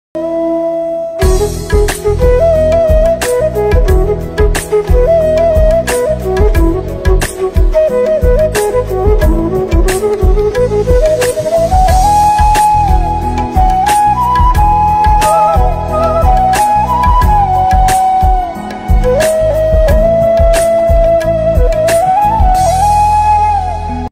Saree Drape | Kalpana Shah| Saree Draping | Sari Draping| Mumbai| Wedding
n a unique draping style, our beautiful bride @aashna_24 radiated purity and virtue in her wedding pheras. The chosen white colour symbolised these qualities, making her stand out with grace and elegance. Isn’t she looking gorgeous? 🌸

Draping dupatta with a veil - kalpanashah @brindashah_love

Outfit- @smitashaofficial
MAU- @shamiitag
Photographer- @blueappleweddings
Location- Courtyard by Marriott, Mahabaleshwar